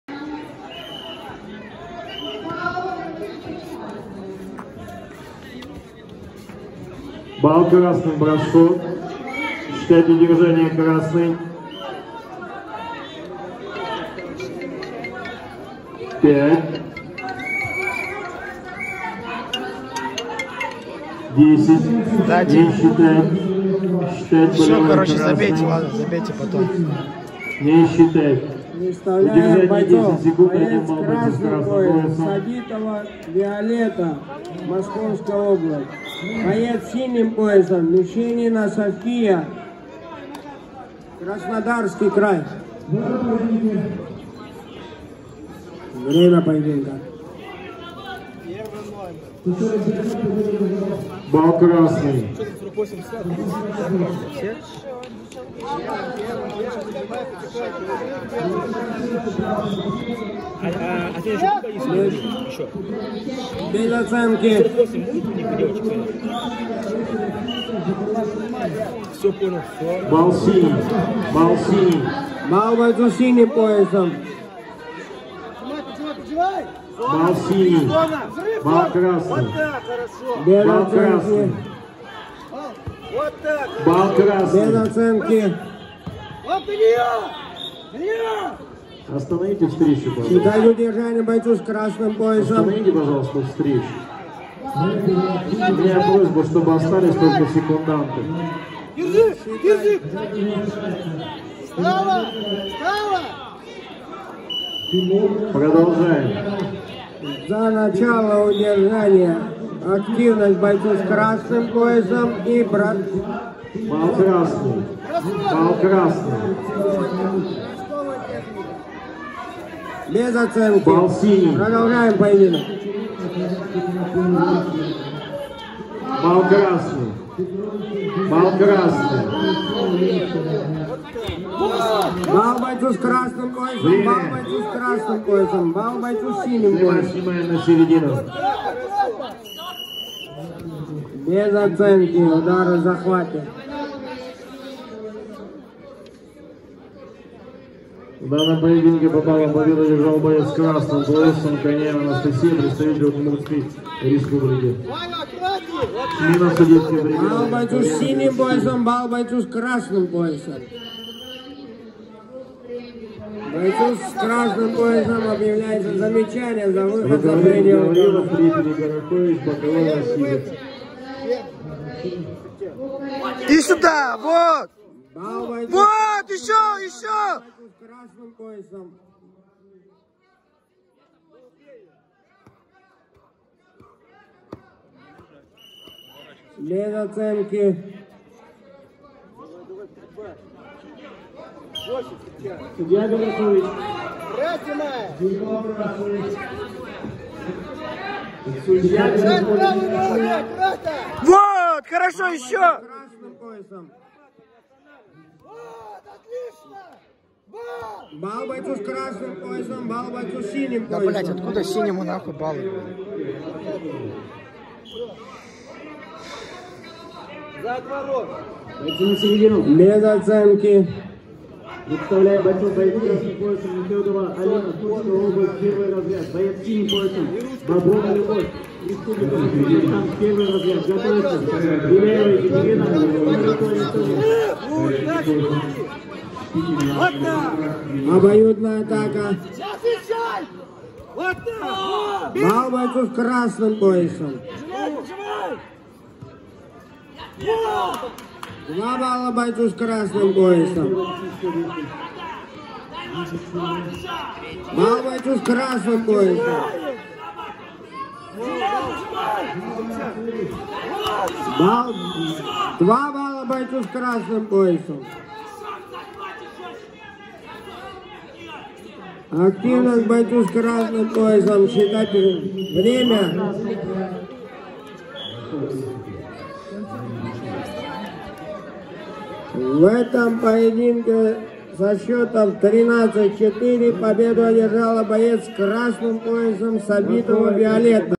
Сабитова Веолетта (красный пояс), 1 бой, клуб ЯРОПОЛК, Первенство России по рукопашному бою,24.02.22
С 21 по 26 февраля 2022 года в г. Орёл прошло Первенство России по рукопашному бою среди юниоров, юношей и девушек от 14 до 21 года.
✅ Спортсменка СПК «ЯРОПОЛК» Сабитова Веолетта представляла на прошедшем турнире сборную команды Московской области и заняла 3 место.

Поздравляем Веолетту с бронзовой медалью, которая, несомненно, стала ярким подтверждением её упорства, активной жизненной позиции и трудолюбия. Желаем ей не останавливаться на достигнутом, стремиться к большему, всегда быть удачливой и успешной.
2. Клуб в яндекс ДЗЕН
3. Канал клуба в РУТУБЕ